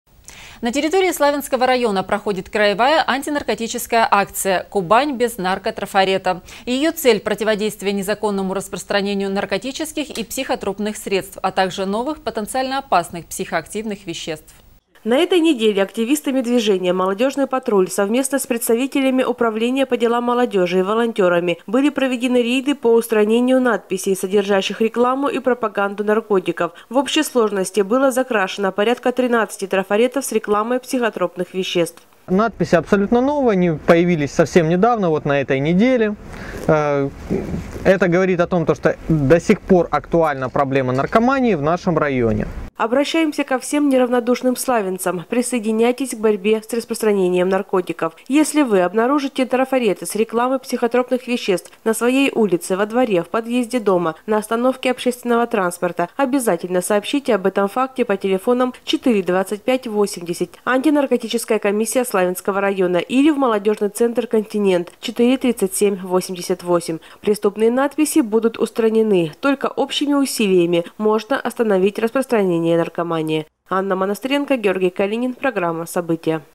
Краевая Антинаркотическая акция “Кубань без наркотрафарета”
На территории Славянского района  проходит краевая Антинаркотическая акция “Кубань без наркотрафарета”. Ее цель — противодействие незаконному распространению наркотических и психотропных средств, а также новых потенциально опасных психоактивных веществ.